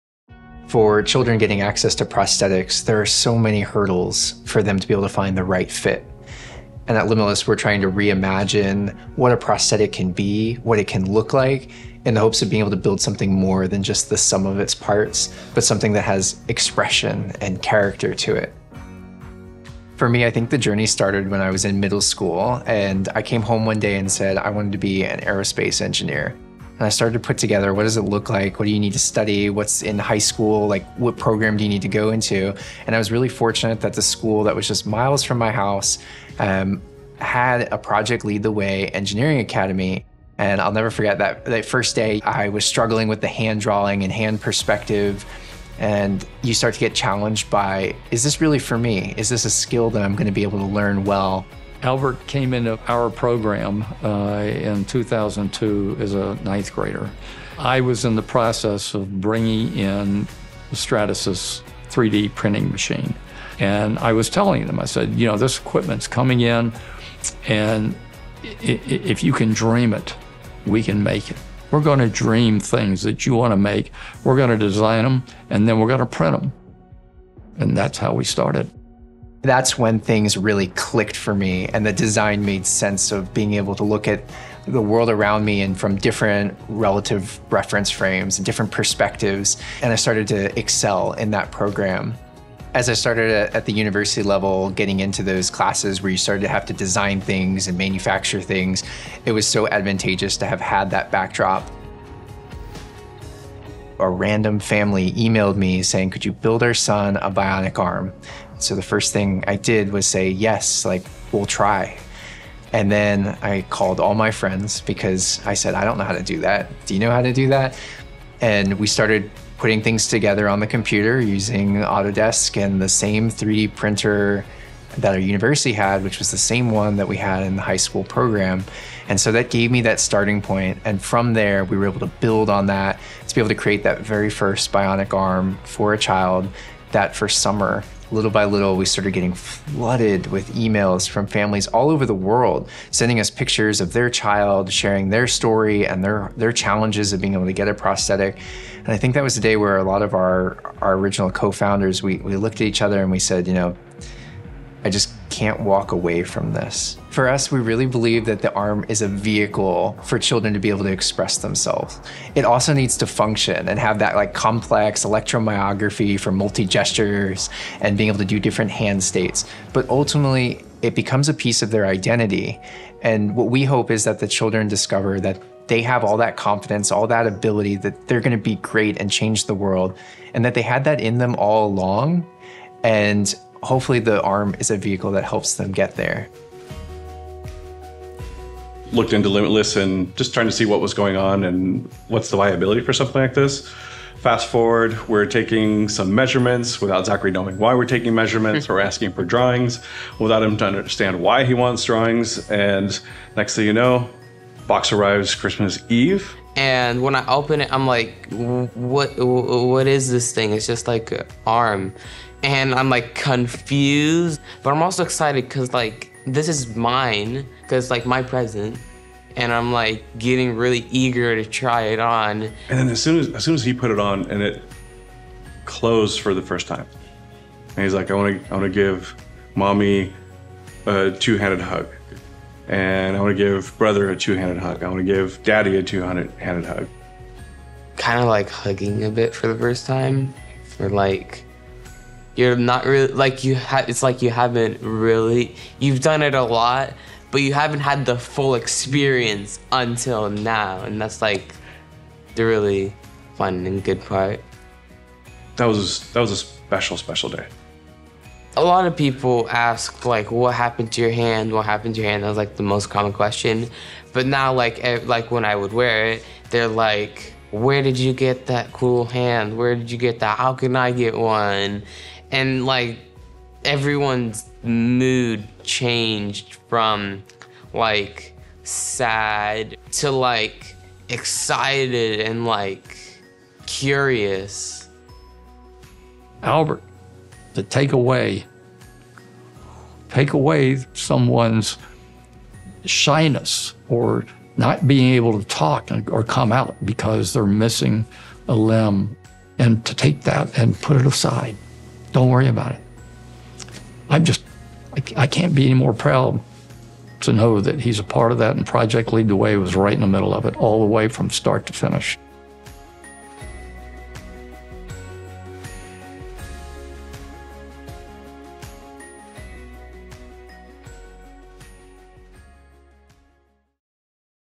PLTW Alumni Making a Difference - Ep. 1 Albert Manero
Being a PLTW alum pays off! At the University of Central Florida, PLTW alums are finding a connection with a fellow PLTW alumnus, Albert Manero, PhD, cofounder of Limbitless Solutions (@Limbitless3D). Albert recruits and hires UCF students, many of them PLTW alums, to work as interns in his office. “PLTW alumni arrive at the university with experience in 3D CAD software, such as Autodesk, which can help accelerate their onboarding,” Albert said.

Albert turned his passion for engineering into a mission to change lives through innovation. Through Project Lead The Way (PLTW) Engineering courses at East Lake High School, Albert discovered his love for design, problem-solving, and hands-on STEM learning. His PLTW experiences helped shape his journey to becoming the CEO and cofounder of Limbitless Solutions, a nonprofit organization creating bionic arms for children in need.

At the University of Central Florida, Albert now recruits and mentors the next generation of STEM innovators: many of them fellow PLTW alumni. As the cofounder of Limbitless Solutions, he hires UCF students to work as interns in his lab, allowing them to apply their classroom knowledge to real-world innovation.

“PLTW alumni arrive at the university with experience in 3D CAD software, such as Autodesk, which can help accelerate their onboarding,” Albert shared. By building on their PLTW foundation in engineering, design, and problem-solving, these students are able to make an immediate impact as interns and future leaders. Albert’s journey comes full circle as he uses his own PLTW experience to create hands-on opportunities that empower students and improve lives.